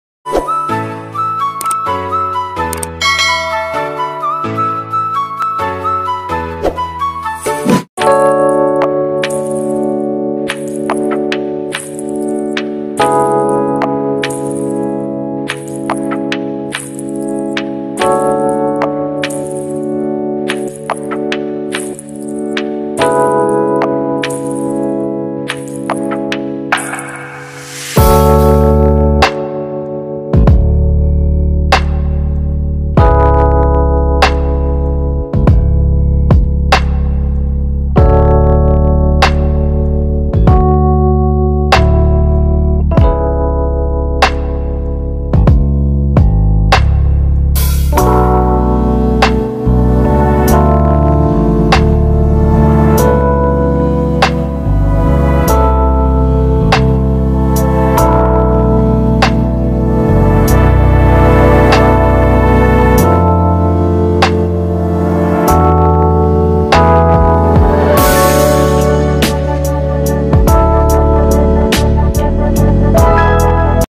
Macbor Rockster 125cc 2022 Classic , Retro, Custom , Cruiser bike, Scrambler?
Macbor Rockster 125 2022, Pang rock n roll na 125cc na motor pogiboyz
credit to the video owner
Macbor Rockster 125 2022
still not available in the philippine
Approximate price in the US is 2599$
price may change depending on the motorcycle dealers